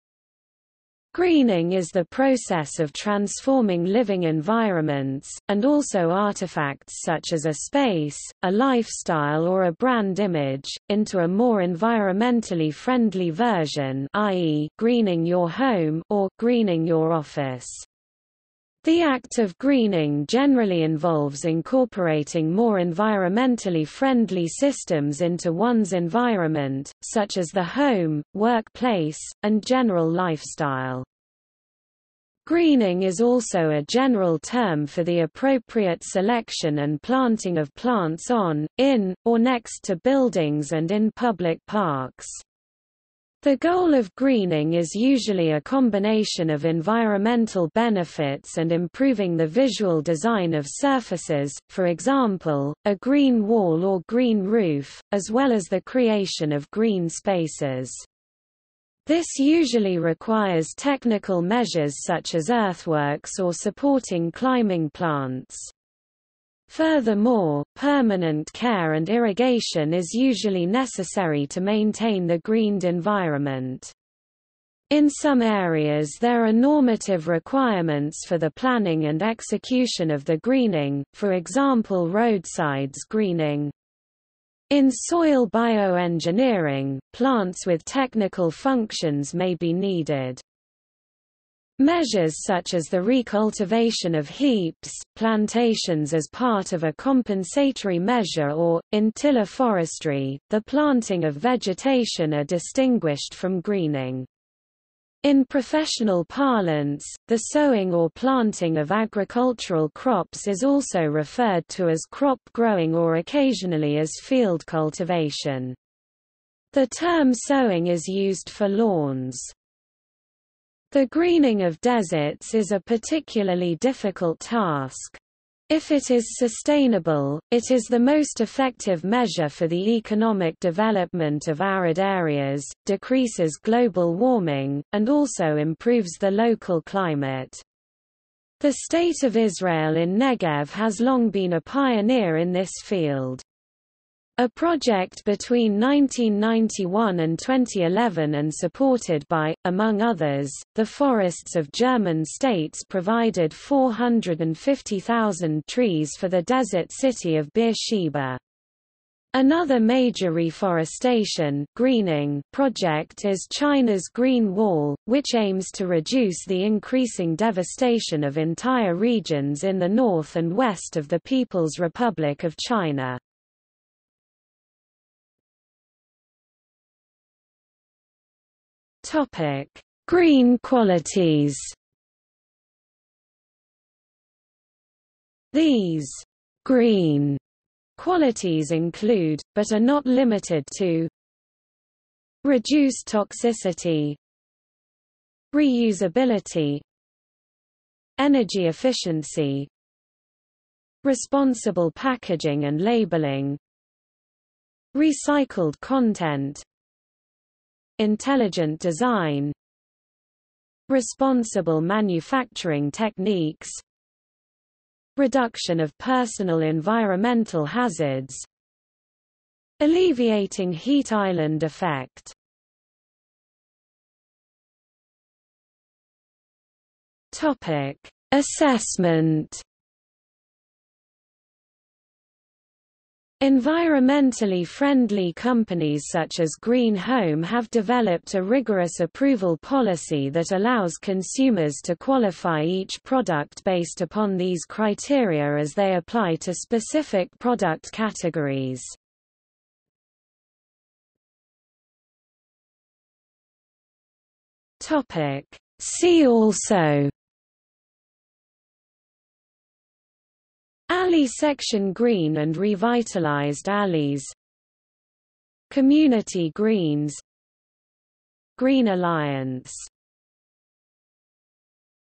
Greening | Wikipedia audio article
00:02:55 1 Green qualities
00:03:43 2 Assessment
00:04:11 3 See also

- Socrates

SUMMARY
Greening is the process of transforming living environments, and also artifacts such as a space, a lifestyle or a brand image, into a more environmentally friendly version (i.e. 'greening your home' or 'greening your office').  The act of greening generally involves incorporating more environmentally friendly systems into one's environment, such as the home, work place, and general lifestyle.

Greening is also a general term for the appropriate selection and planting of plants on, in, or next to buildings and in public parks. The goal of greening is usually a combination of environmental benefits and improving the visual design of surfaces, for example, a green wall or green roof, as well as the creation of green spaces. This usually requires technical measures such as earthworks or supporting climbing plants. Furthermore, permanent care and irrigation is usually necessary to maintain the greened environment. In some areas there are normative requirements for the planning and execution of the greening, for example roadsides greening. In soil bioengineering, plants with technical functions may be needed.
Measures such as the recultivation of heaps, plantations as part of a compensatory measure or, in tiller forestry, the planting of vegetation are distinguished from greening. In professional parlance, the sowing or planting of agricultural crops is also referred to as crop growing or occasionally as field cultivation. The term sowing is used for lawns.
The greening of deserts is a particularly difficult task. If it is sustainable, it is the most effective measure for the economic development of arid areas, decreases global warming, and also improves the local climate. The state of Israel in Negev  has long been a pioneer in this field. A project between 1991 and 2011 and supported by, among others, the forests of German states provided 450,000 trees for the desert city of Beersheba. Another major reforestation (greening) project is China's Green Wall, which aims to reduce the increasing devastation of entire regions in the north and west of the People's Republic of China.